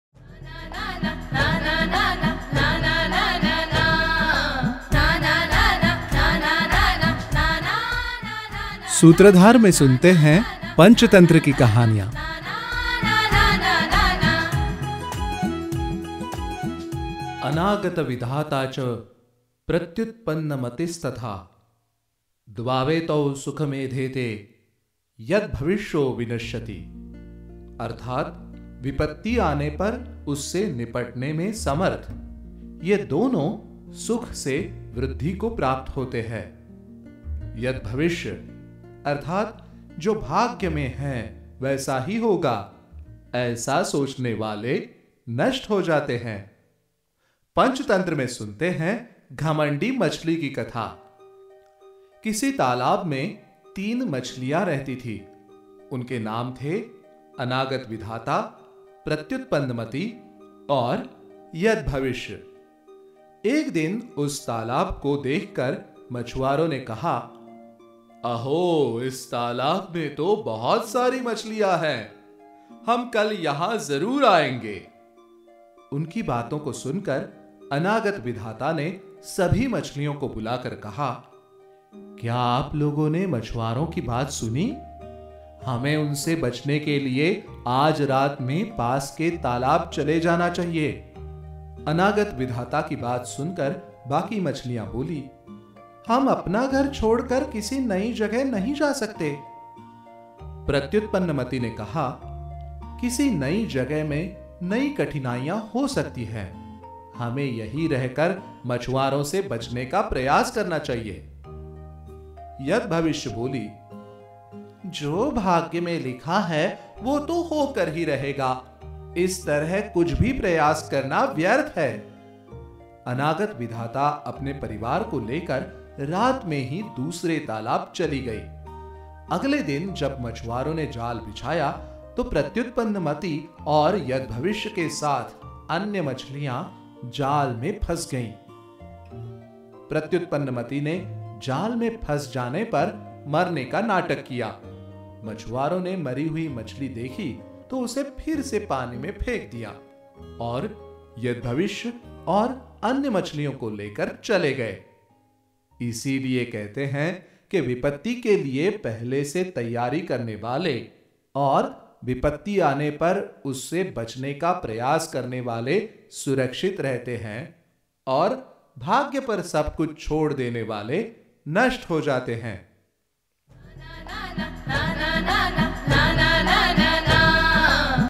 तीन मछलियाँ | बाल कहानी | पंचतंत्र | Three Fishes | Moral Story | Panchtantra | Hindi
Learn the story of prior planning and presence of mind from this moral story from Panchtantra.